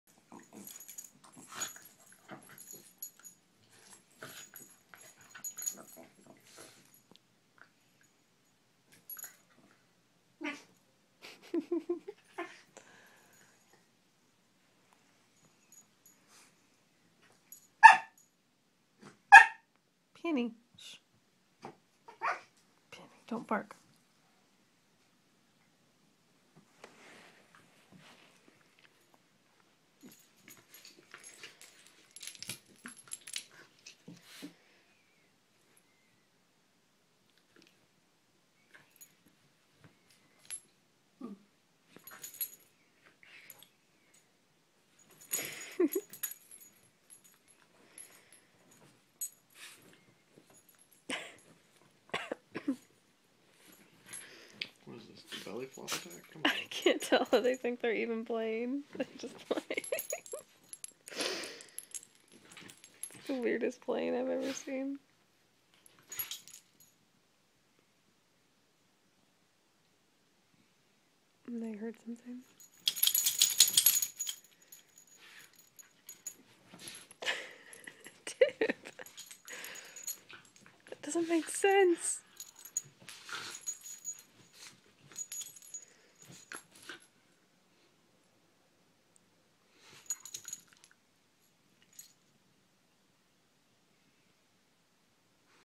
Shiba Inu vs Corgi.
Dib and Penny playing in their own way. Gaz and Bella seem to be doing the dog equivalent of shaking their heads.